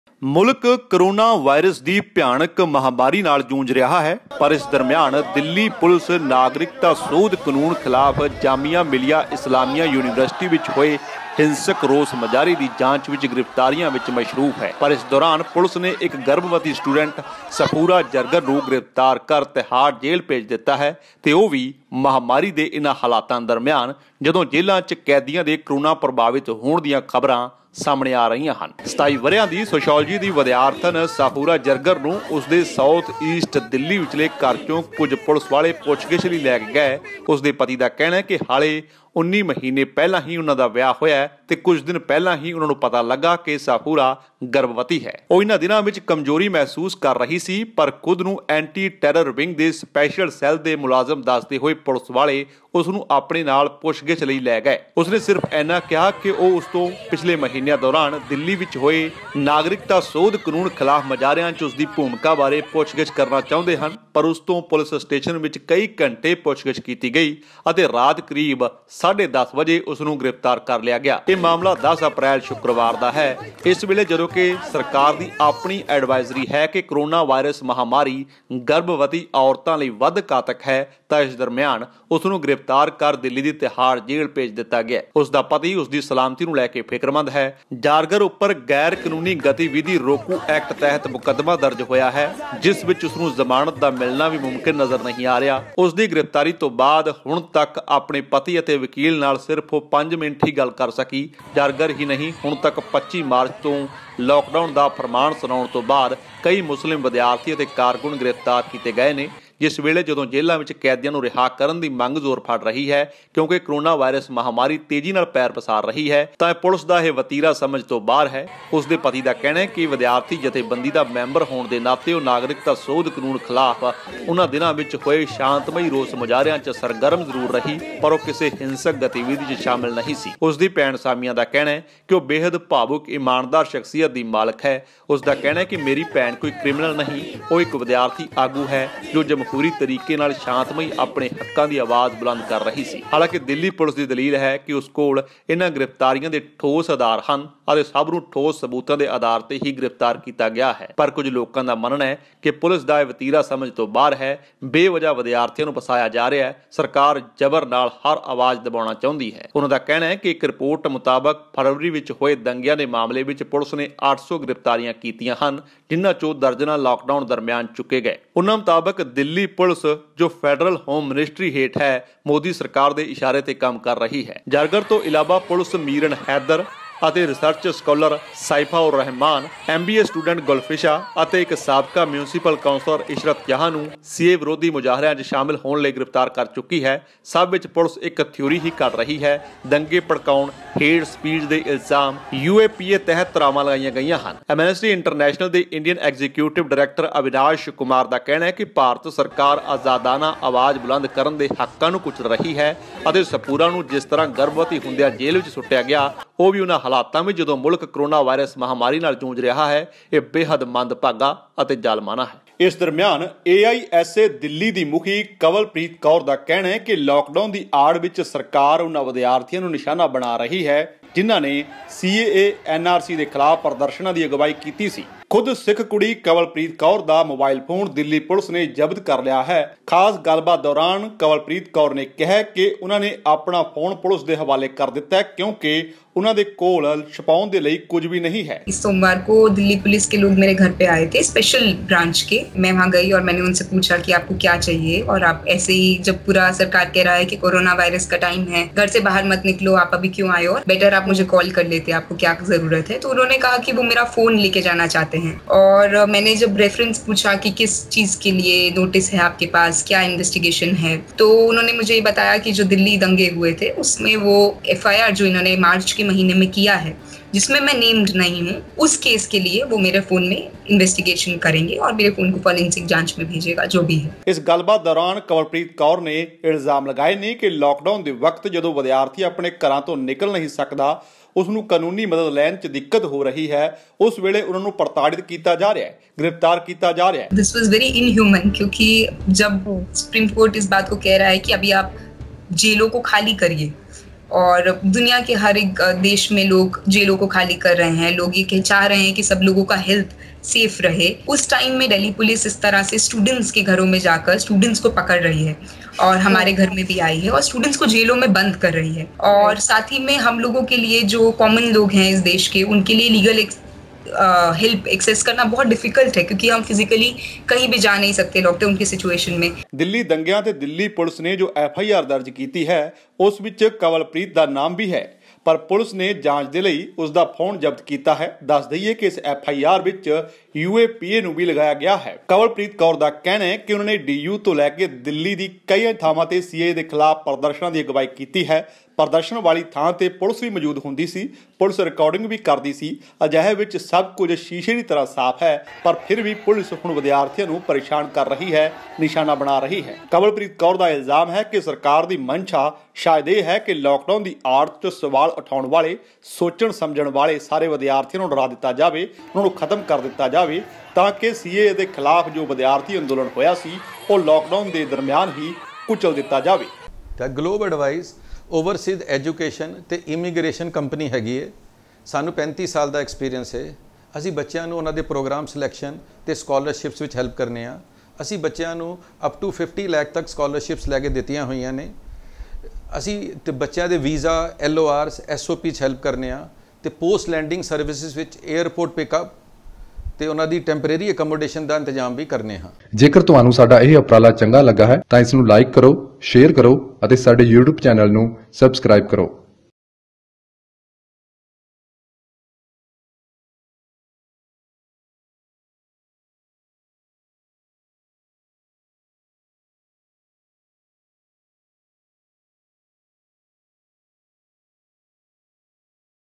CORONA ਦਰਮਿਆਨ ਗਰਭਵਤੀ MUSLIM ਕੁੜੀ ਨੂੰ UAPA ਲਾ ਭੇਜਿਆ ਜੇਲ੍ਹ, AISA ਮੁਖੀ KAWALPREET KAUR ਦਾ ਵਿਰੋਧ || SNE
Video footage : The Tribune,  Jamun  Kumar Abhinav, Quint Hindi